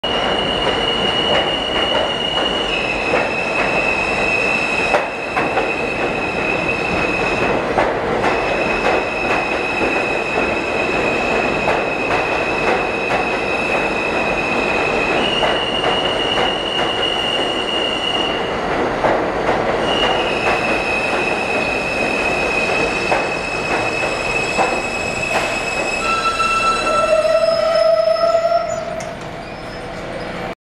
F train at DeKalb Avenue
Manhattan-bound F train of R160As arriving at DeKalb Avenue. Due to construction, F trains were rerouted via Manhattan Bridge, 4th Avenue (local northbound and express southbound) and West End between Broadway-Lafayette Street and Coney Island-Stillwell Avenue.